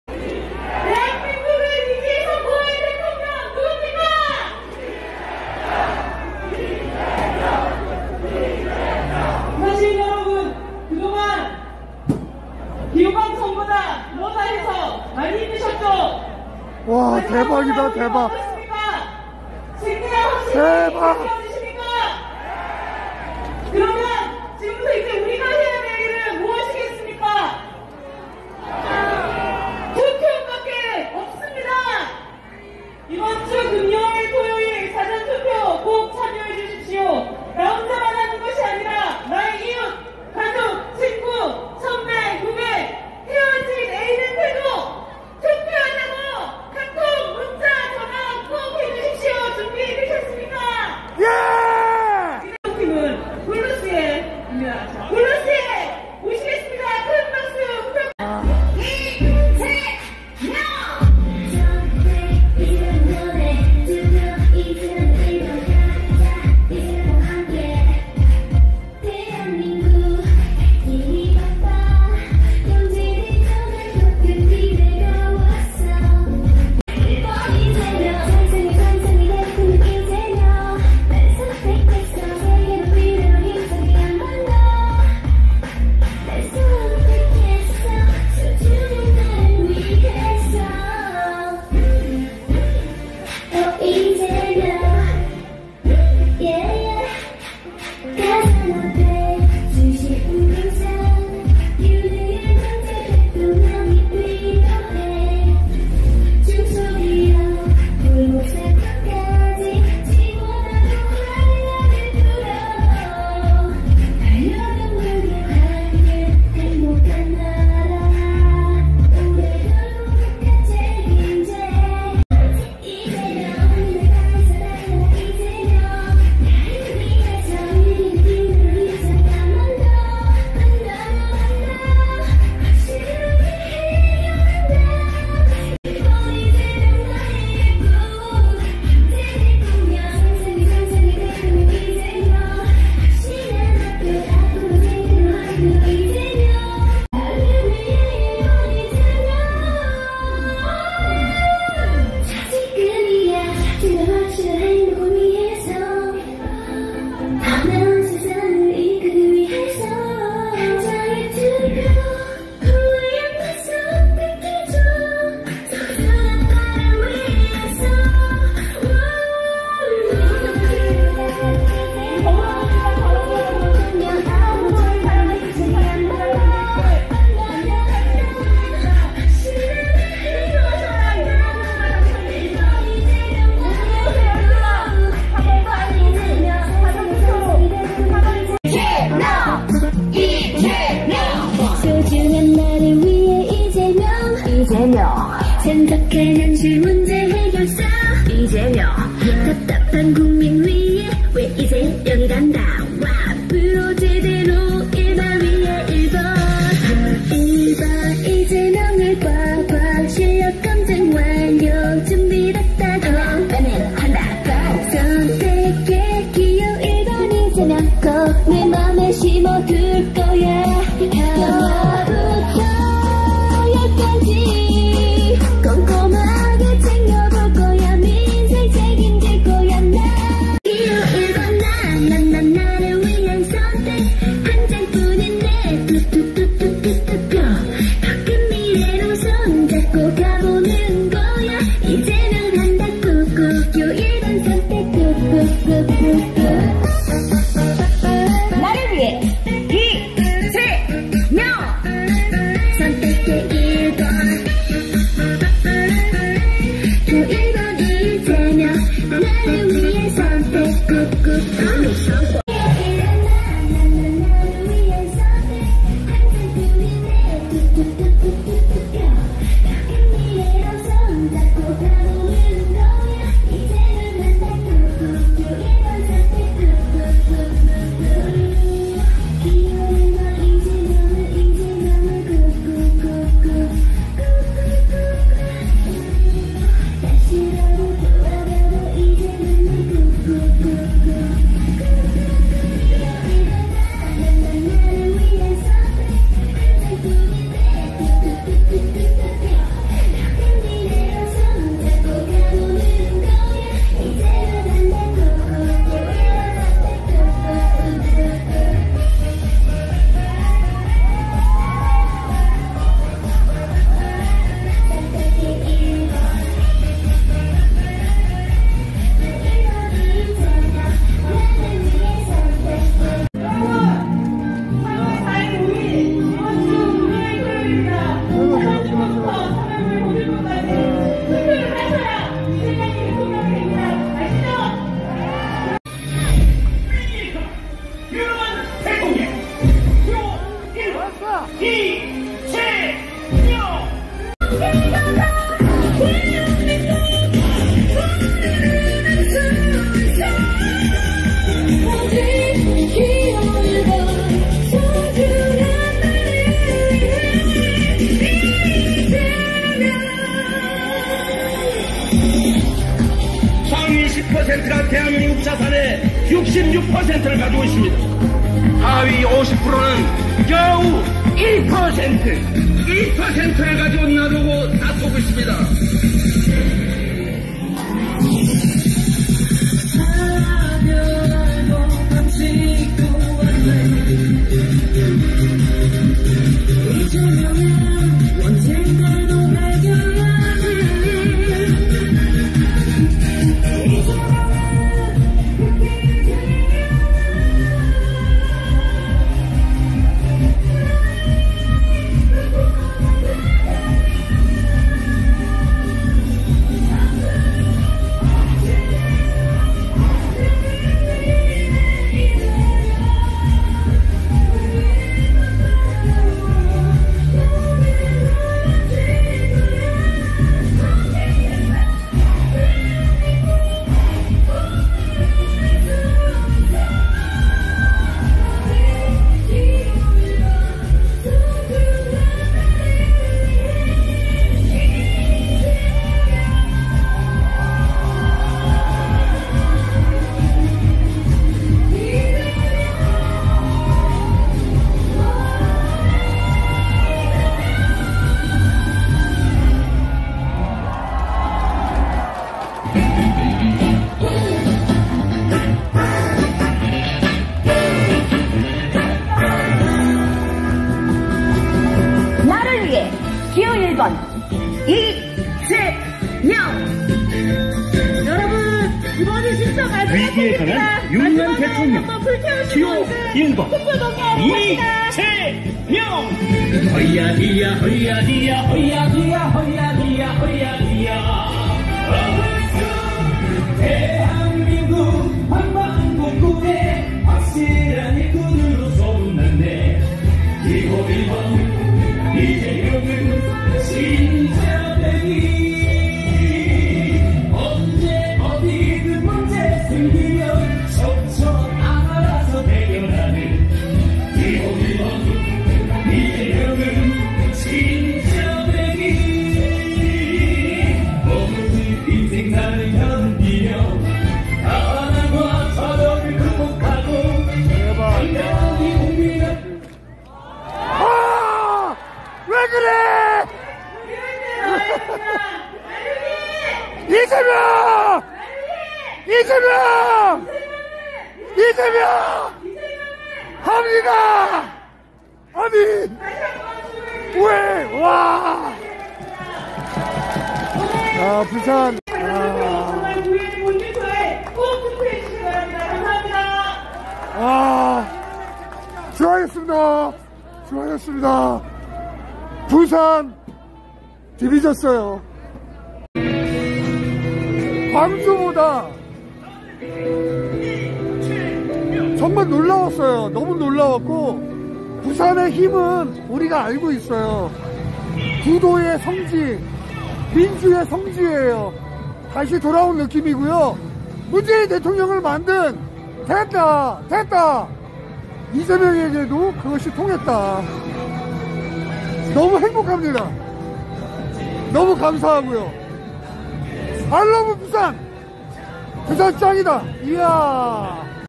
부산유세 뒷풀이대박! 블루씨 유세단!!!
이재명 후보의 유세가 끝나고 난뒤,  엄청난 뒷풀이
위로와 용기의 살풀이
부산시민 여러분 최고